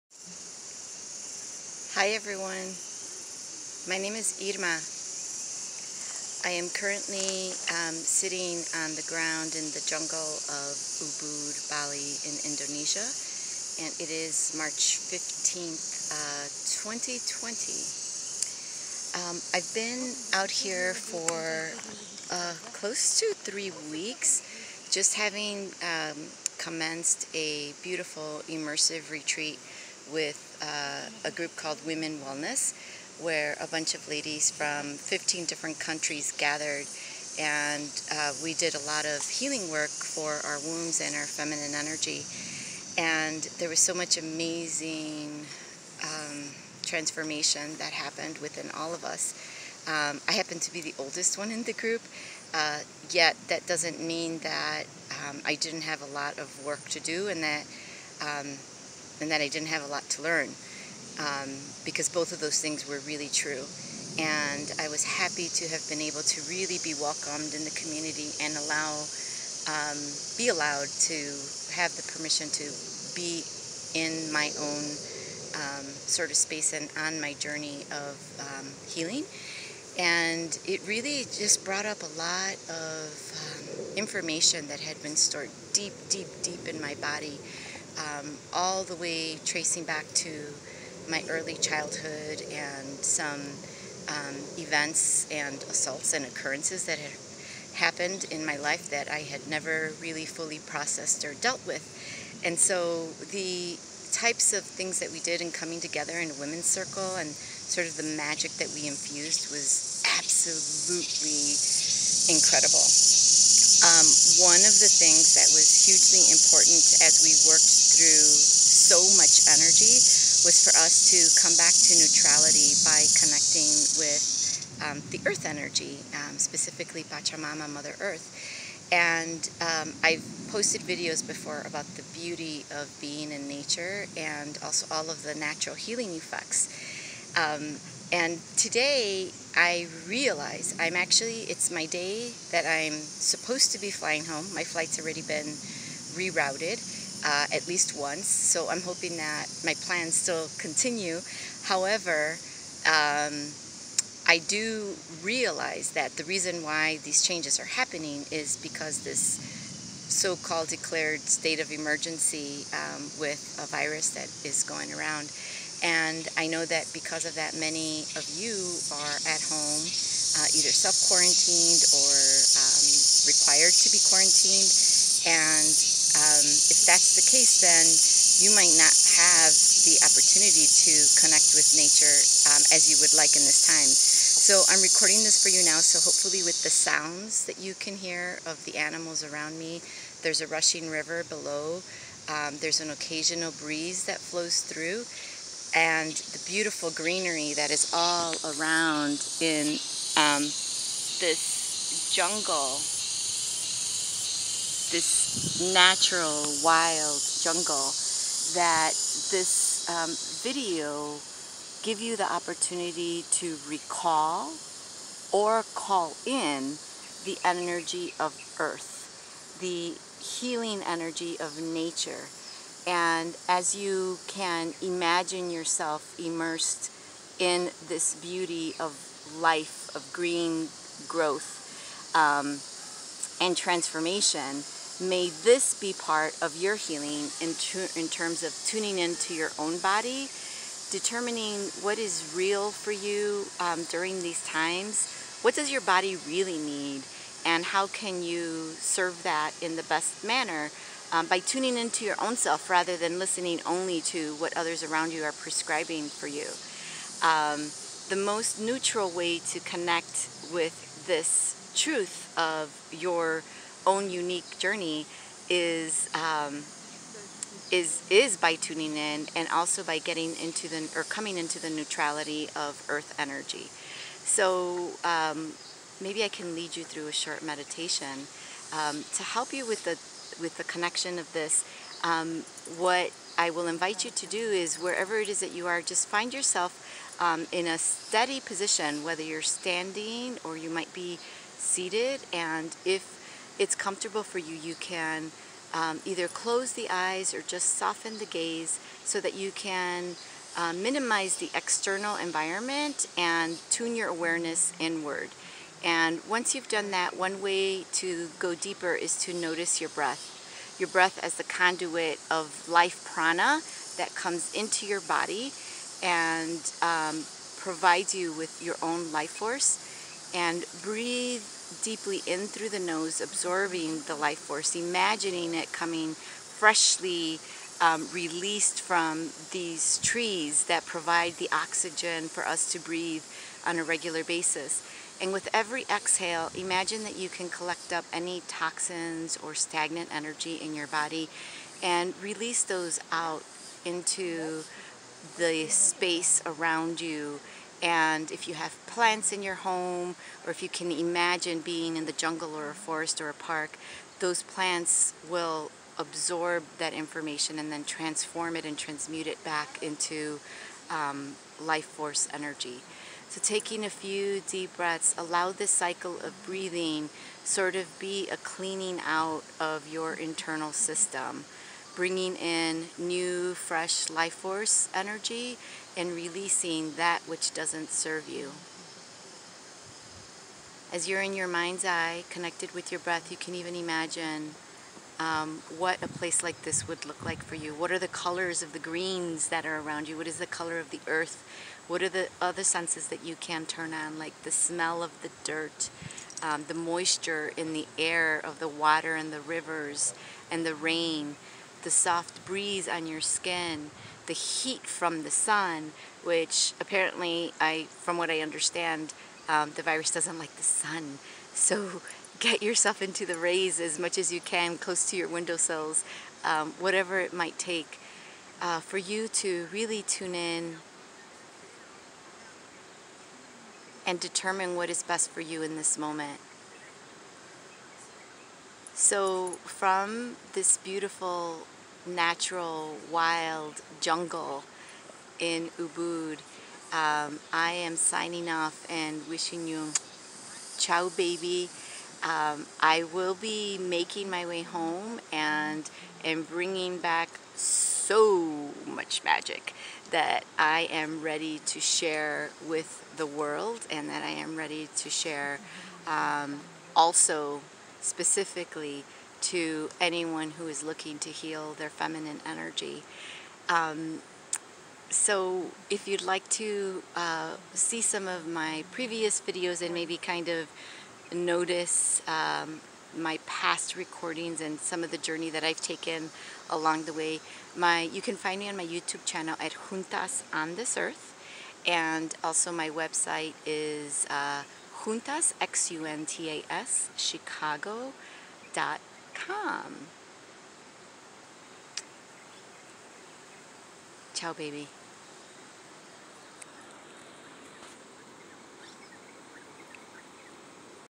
Match to a healing earthenergy frequency with imagination. Jungle Ubud, Bali, Indonesia 3.15.20
When you think you don't have access to healing energy. Turn to nature and you'll remember your true nature and ability to heal.